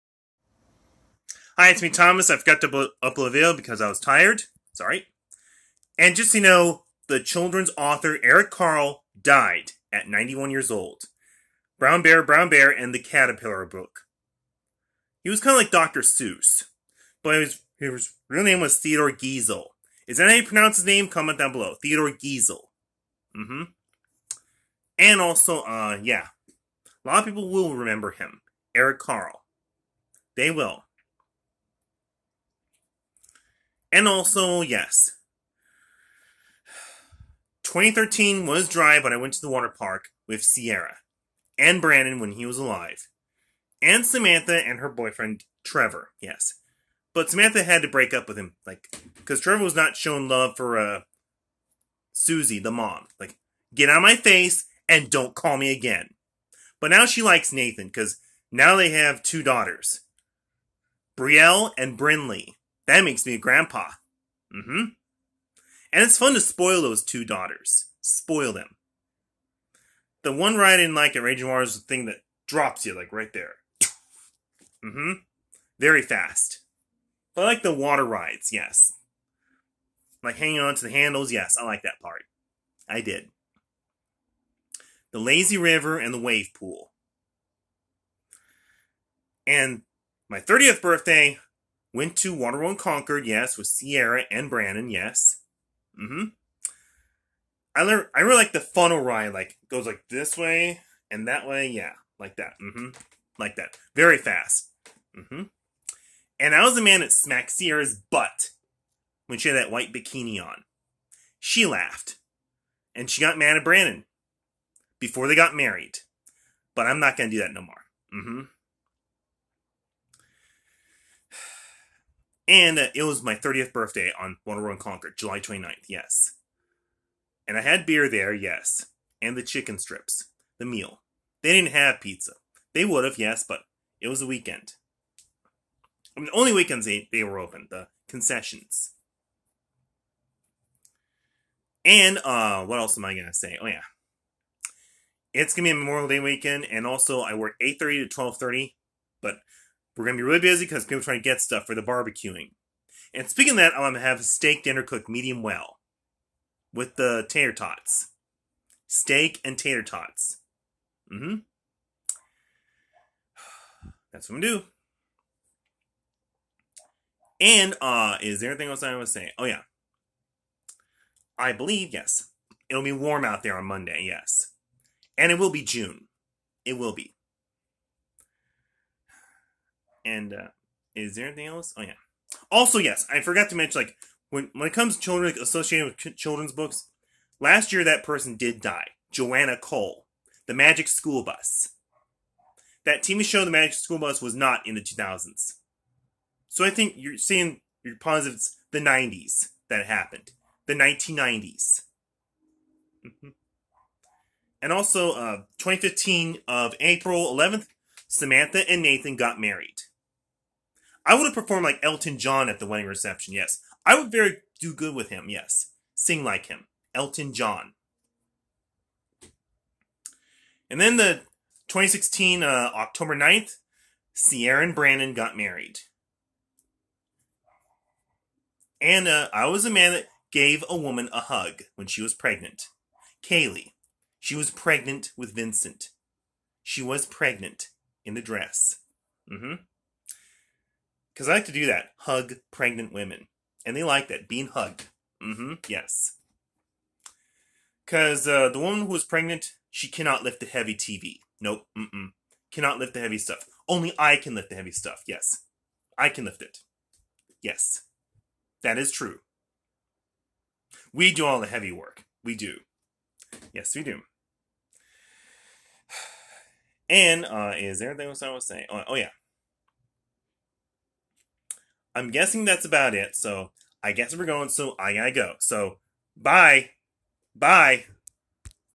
Thomas Murphy knows that Eric Carle died at 91 years old similar to Dr. Seuss.
Thomas Murphy knows that Eric Carle died at 91 years old. He did brown bear brown bear and the caterpillar book. Eric Carle was like Dr. Seuss and his real name is Theodore Geisel. 2013 was dry but on his birthday in 2013,Thomas,Sierra,brandon,Samantha,Trevor and his mom went to Raging Waters but Samantha had to break up with Trevor because he did no show the love for Samantha's mom when she died and Samantha told Trevor Get out of my face and don't call me again. Samantha now likes Nathan and they have two daughters. The names are Brielle and Brynnlee so that makes Thomas a grandpa and he likes to spoil them. The one ride at Raging Waters that Thomas doesn't like is the door that opens below your feet, you fall. Thomas likes the tube rides better and the lazy river and the wave pool. Thomas's 30th birthday in 2014 is Waterworld Concord with Sierra,Brandon before they got married and his mom. Thomas likes the funnel ride and he was the man that smacked Sierra's butt when she had a white bikini on and she laughed but got mad at Brandon and he doesn't do that no more. Thomas had beer and chicken strips for lunch at the Waterpark. It will be hot on memorial day but Thomas has a short shift. It will be busy because people are getting stuff for BBQ. Thomas will have steak cooked Medium-well and tater tots tonight and memorial day dinner. Joanna Cole is the author for The magic School Bus has died in 2020. The magic School Bus tv show was not in the 2000s but early 1990s. April 11,2015 is when Samantha and Nathan got married but Thomas wishes he could perform like Elton John at the reception. October 9,2016 is when Sierra and Brandon got married and Thomas did hug a woman named Kaylee who wore a dress and she was pregnant with Vincent. Thomas enjoys hugging pregnant women. When Kaylee was pregnant,she couldn't lift heavy furniture but Thomas can lift heavy stuff. Next video is May 30.